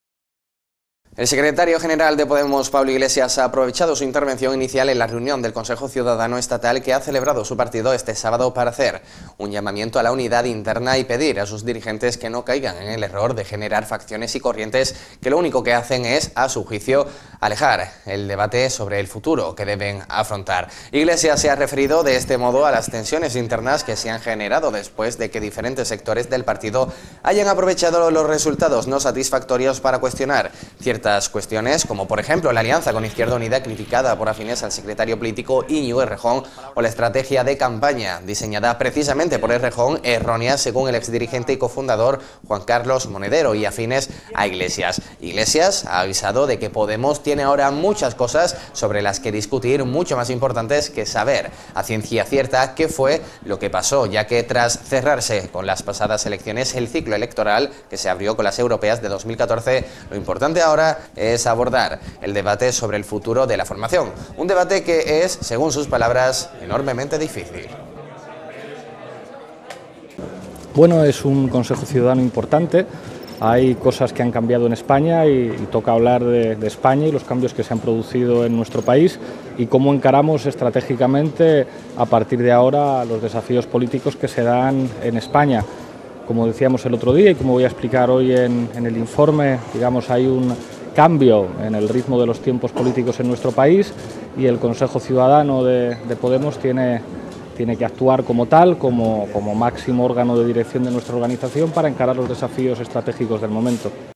Málaga 24h TV - Iglesias llama al orden e insta a los dirigentes de Podemos a no generar "facciones"
El secretario general de Podemos, Pablo Iglesias, ha aprovechado su intervención inicial en la reunión del Consejo Ciudadano Estatal que ha celebrado su partido este sábado para hacer un llamamiento a la unidad interna y pedir a sus dirigentes que no caigan en el error de generar "facciones" y "corrientes", que lo único que hacen, a su juicio, es alejar el debate sobre el futuro que deben afrontar.

Iglesias se ha referido de este modo a las tensiones internas que se han generado después de que diferentes sectores del partido hayan aprovechado los resultados "no satisfactorios" para cuestionar ciertas cuestiones, como por ejemplo, la alianza con Izquierda Unida, criticada por afines al secretario Político, Íñigo Errejón, o la estrategia de campaña diseñada precisamente por Errejón, errónea según el exdirigente y cofundador Juan Carlos Monedero y afines a Iglesias.

Iglesias ha avisado de que Podemos tiene ahora "muchas cosas sobre las que discutir mucho más importantes que saber a ciencia cierta qué fue lo que pasó", ya que, tras cerrarse con las pasadas elecciones el "ciclo electoral" que se abrió con las Europeas de 2014, lo importante ahora es abordar el debate sobre el futuro de la formación; un debate que es, según sus palabras, "enormemente difícil".

English: The secretary general of Podemos, Pablo Iglesias, has used his opening remarks at the meeting of the Citizens' Council State which held its party on Saturday to call on the internal drive and ask their leaders do not fall into the error of generating " factions "and" streams "that all they do, in his opinion, is to remove the debate on the future they face.

Iglesias has referred in this way to internal tensions that have arisen after different sectors of the party have taken advantage of the results "unsatisfactory" to question certain issues, such as the alliance with United Left, criticized by allied Secretary political, Inigo Errejón, or the campaign strategy designed precisely for Errejón, erroneous as the former leader and co-founder Juan Carlos Monedero and related churches.

Iglesias has warned that we now have "a lot to argue about much more important than knowing for sure what had happened," because, after closing the last elections the "electoral cycle" which opened with the European 2014, the important thing now is to address the debate.